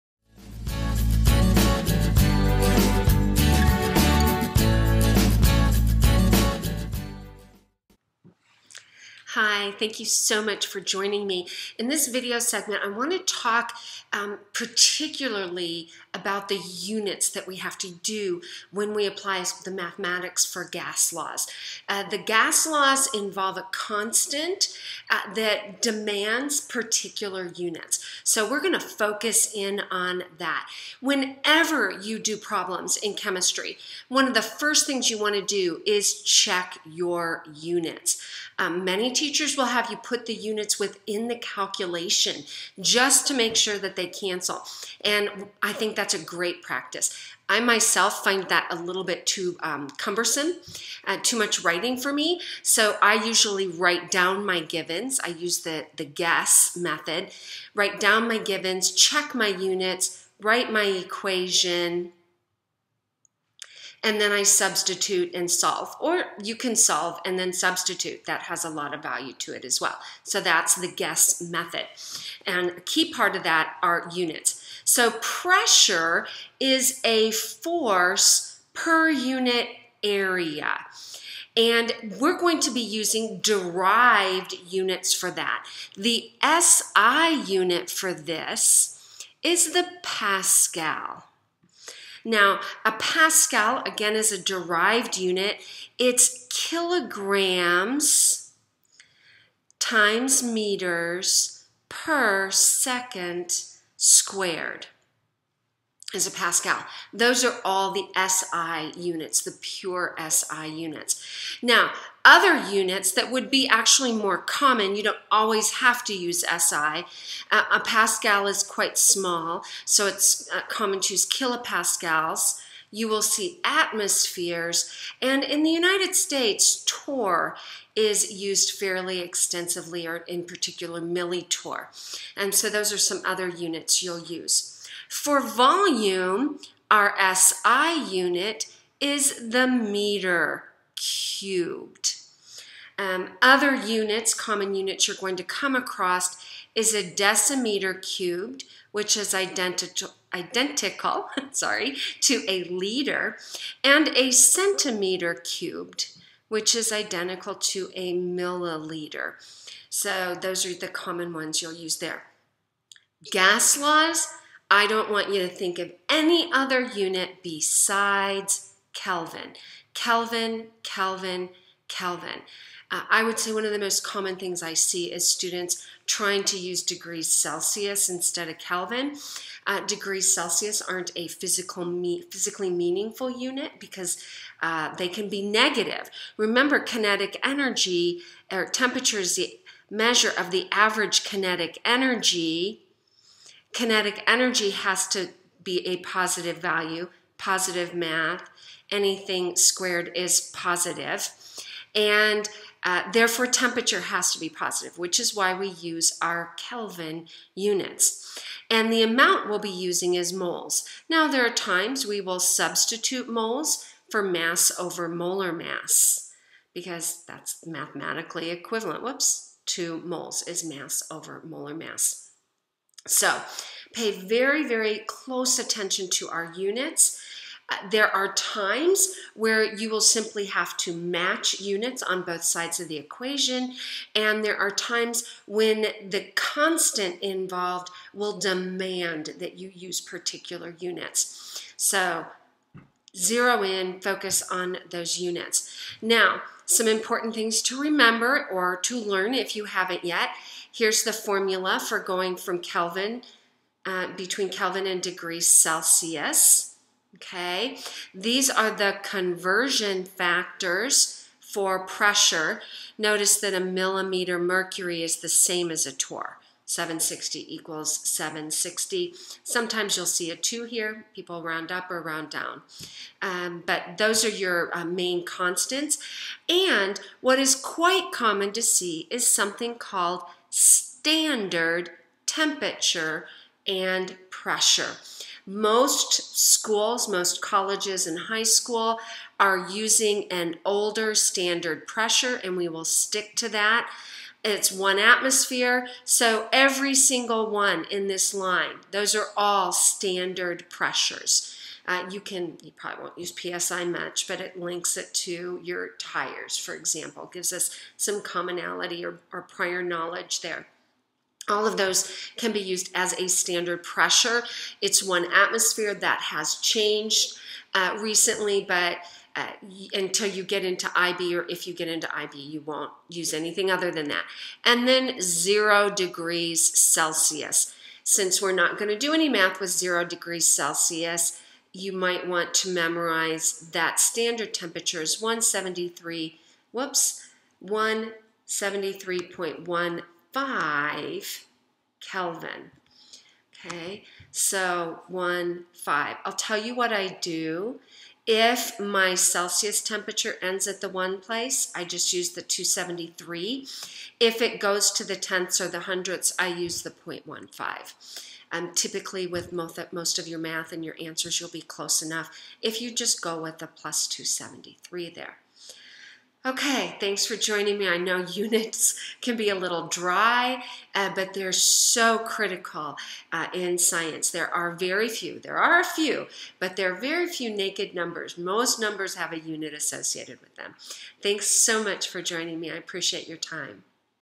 Doc Dena Chemistry 3.4.A Units for Gas Laws
Series of videos discussing gas behavior conceptually and mathematically.  Boyle’s Law, Charles’ Law, Gay-Lussac’s Law, Avogadro’s Law, Dalton’s Law, Ideal Gas Law, and Graham’s Law are included.  The integration of stoichiometry and gas law calculations is addressed.  Non-Ideal gas behavior is briefly evaluated.  All of the videos are anchored in the Kinetic Molecular Theory of gas behavior. @chemistrywithdocdena7986